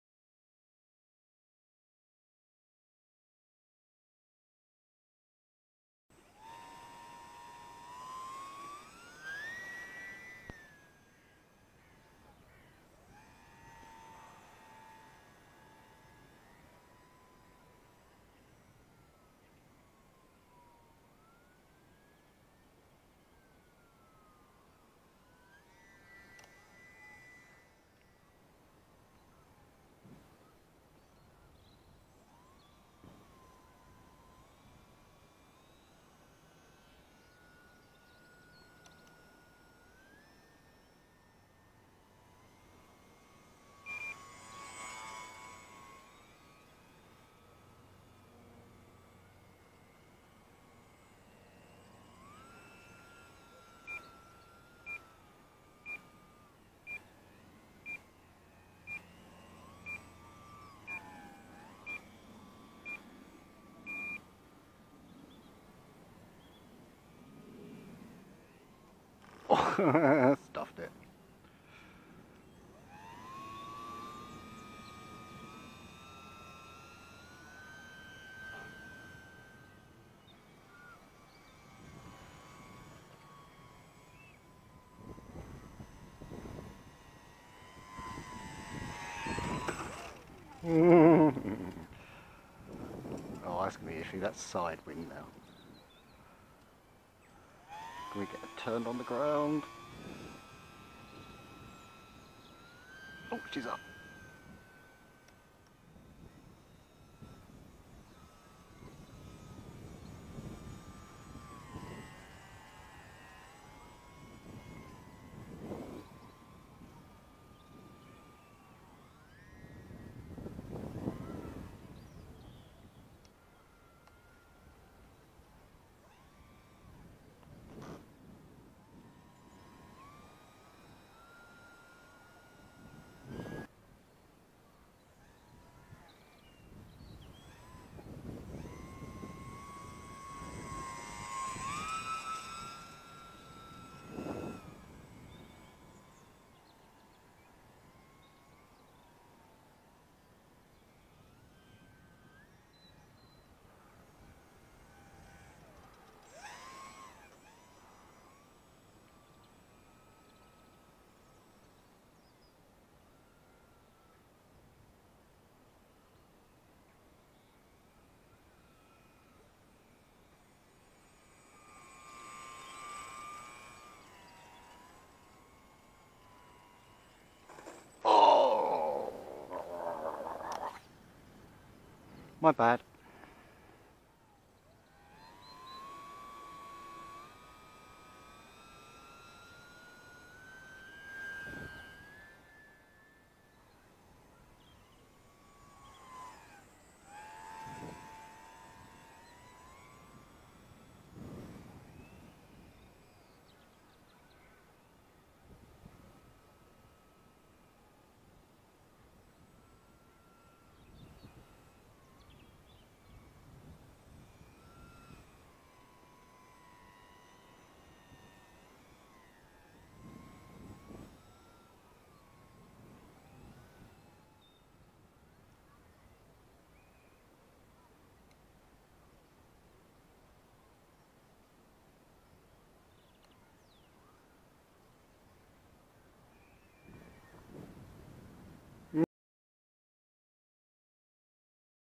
UMX Turbo Timber in 4k.
All my other airframes (apart from the BushRanger build) are belly landers, so this whole thing about touch and go circuits needs a bit of practice.

Filmed in 4k as a bit of a test to see if it is worth the upload time v my normal 1080p uploads from either 1080,2.5 or 4k raw footage.

Be safe.